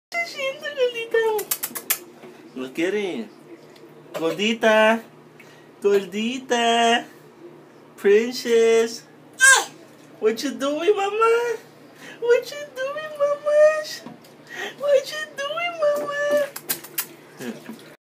Bath time with her frogs!
Daliah enjoys a fresh bath while playing with her frogs 8 mo. (May 2013)